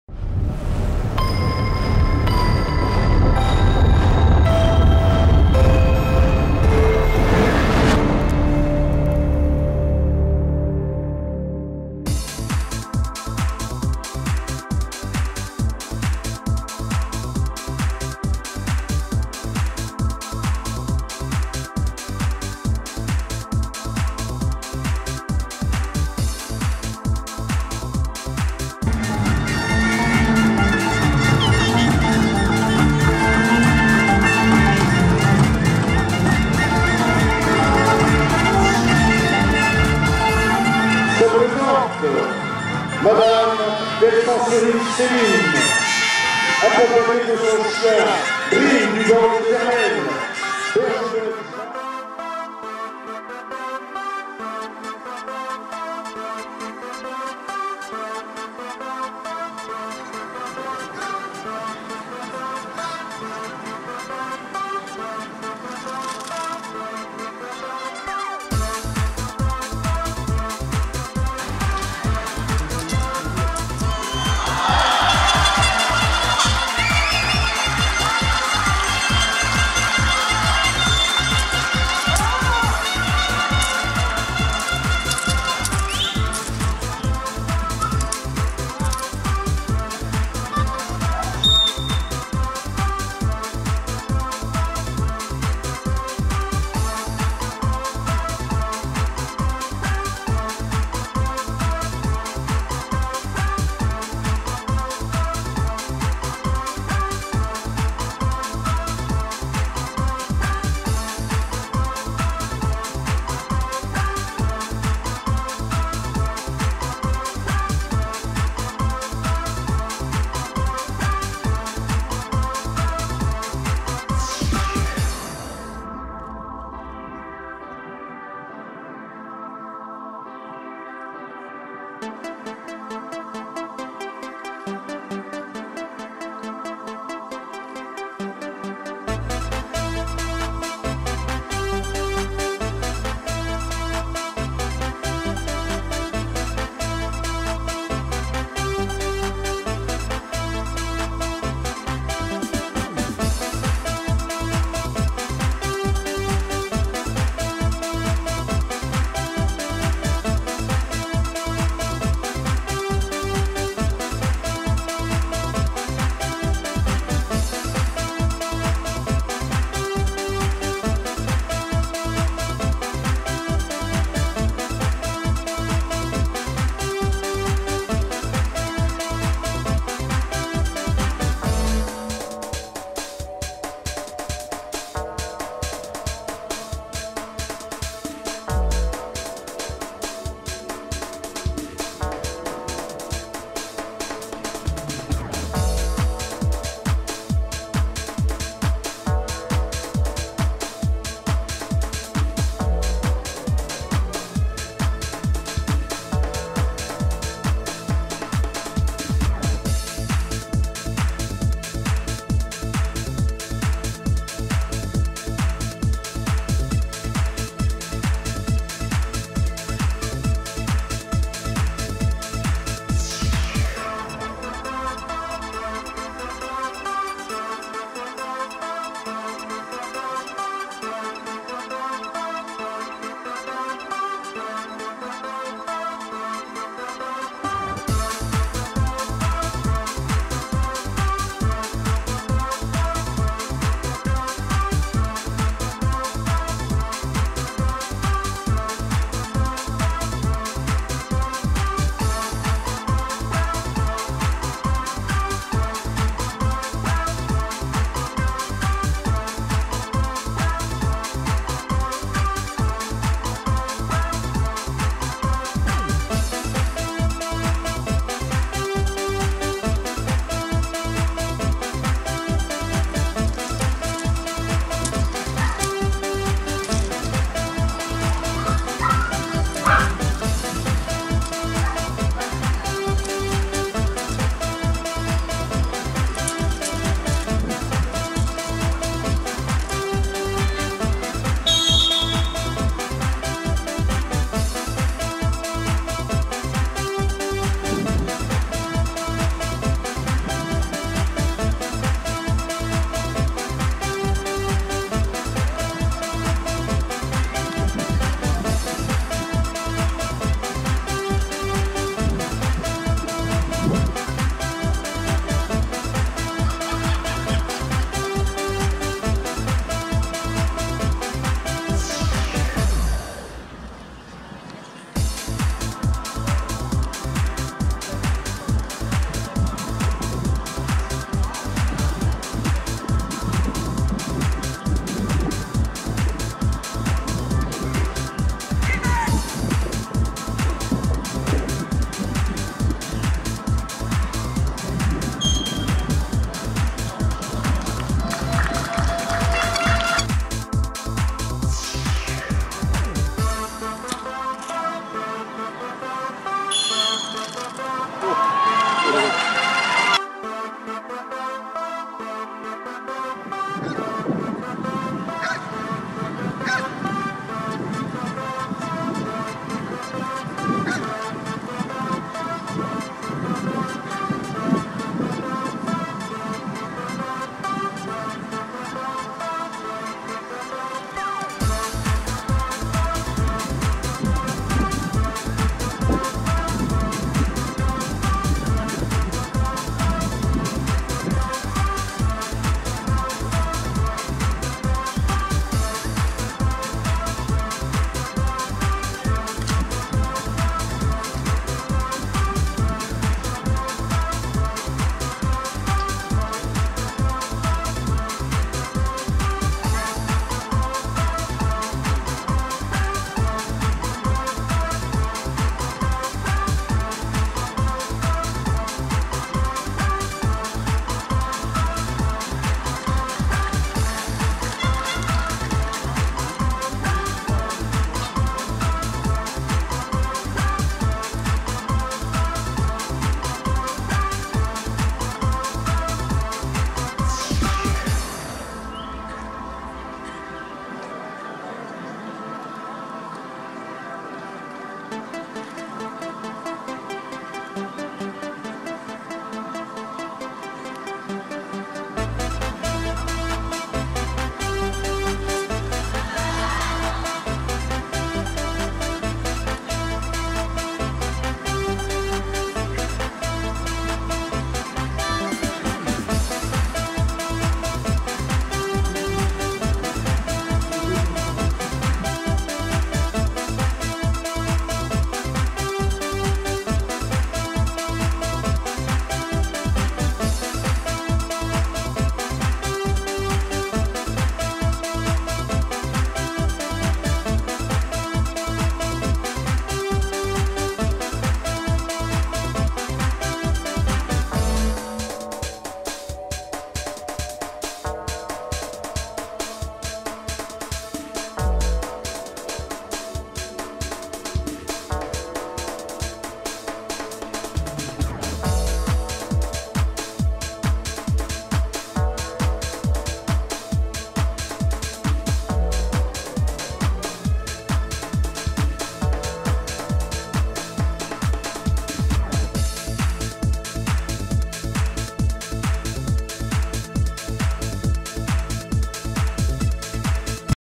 2015 - Championnat de France Ring à Domérat Dream du Banc des Hermelles - Céline Delcenserie
2015 - Championnat de France Ring à Domérat Dream du Banc des Hermelles - Céline Delcenserie
Juges : Delpech Jean-Claude, Touy Philippe et Volle Eric
HA : Axel Villain, Johann Verry, remplaçant Sofiane Tarefet